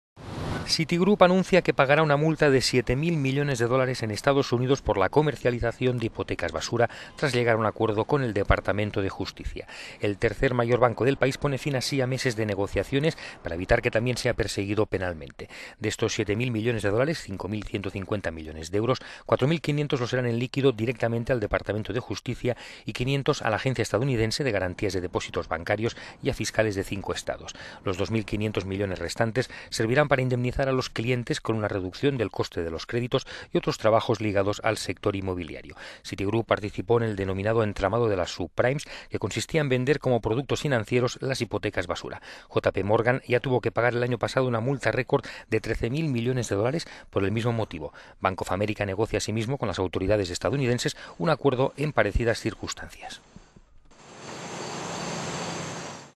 Citrigroup acepta pagar 7.000 millones de dólares de multa en EEUU por las 'subprimes' - corporate
Citigroup anuncia que pagará una multa de 7.000 millones de dólares en Estados Unidos por la comercialización de hipotecas basura, tras llegar a un acuerdo con el departamento de Justicia. El tercer mayor banco del país pone fin así a meses de negociaciones para evitar que también sea perseguido penalmente. De estos 7.000 millones de dólares - 5.150 millones de euros -, 4.500 lo serán en líquido directamente al departamento de Justicia y quinientos a la agencia estadounidense de garantías de dep...